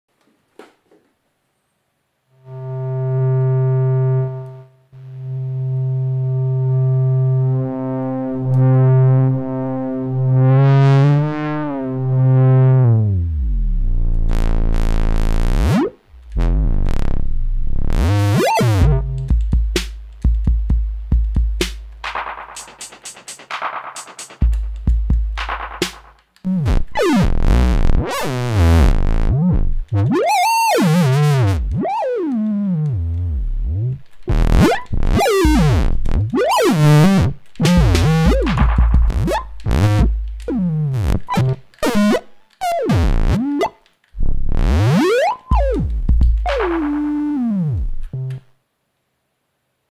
FH-1 firmware v2 - gamepad demo
Direct connection of PlayStation4 controller to modular synth via the Expert Sleepers FH-1 - a new feature of the v2 firmware.

The DualShock4 is plugged directly into the FH-1 via a USB cable. The FH-1 and its FHX-1 expander are outputting various CVs and triggers driven from the controller's joysticks and buttons.